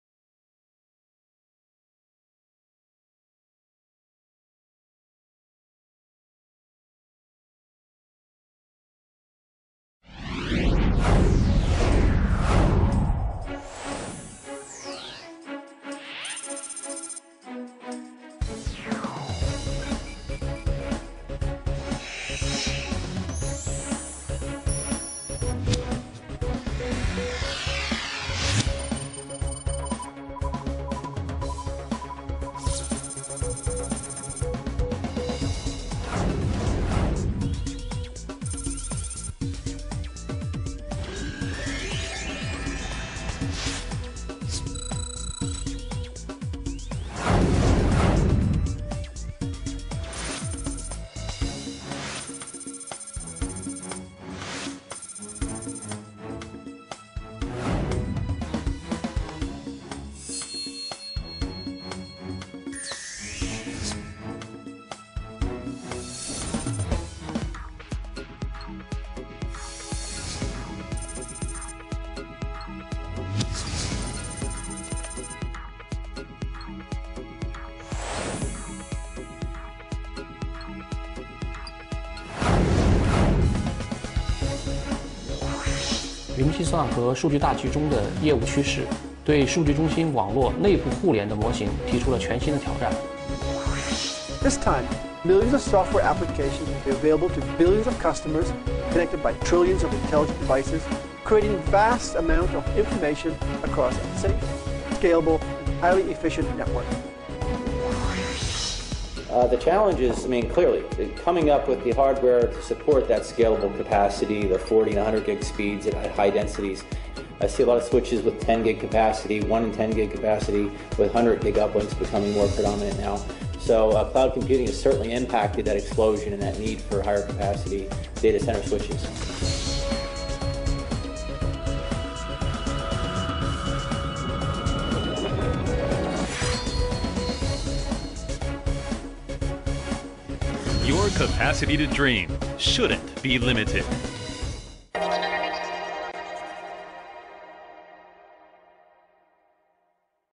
The Answer of Cloud Computing Huawei CloudEngine 12800
Huawei CloudEngine series switches are high-performance cloud switches designed for next-generation data centers and high-end campus networks. The whole series include CloudEngine 12800 flagship core switches with the highest performance in the world and CloudEngine 6800/5800 high-performance case-shaped switches (for 10GE/GE access). CloudEngine series uses Huawei's new software platform VRP8 and support abundant data center and campus network service features.

Huawei launched the Cloud Fabric solution for next-generation cloud-computing data centers. This solution combines CloudEngine series (CE for short) switches with transmission, routing, security and network management products of Huawei to help customers build a stable network architecture supporting development for the next 10 years.